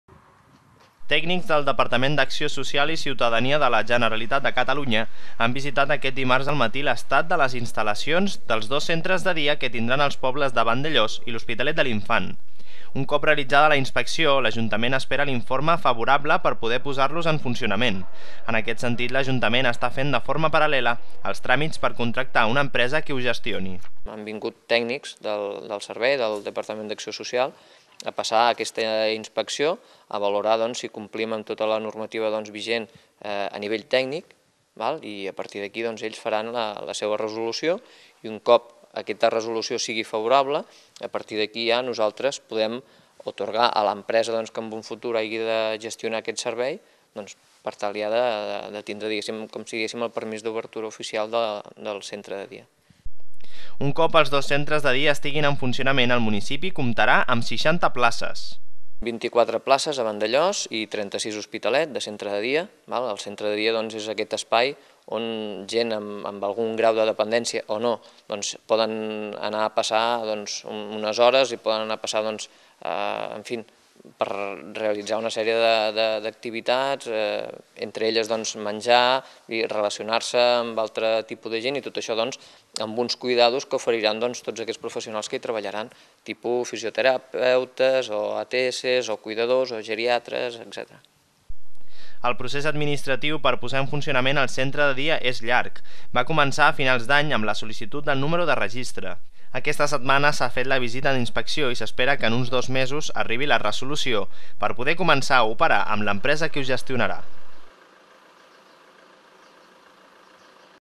El centre de dia més a prop
Tècnics del departament dacció social i ciutadania de la Generalitat han visitat aquest dimarts al matí lestat de les instal•lacions dels dos centres de dia que tindran els pobles de Vandellòs i lHospitalet de lInfant.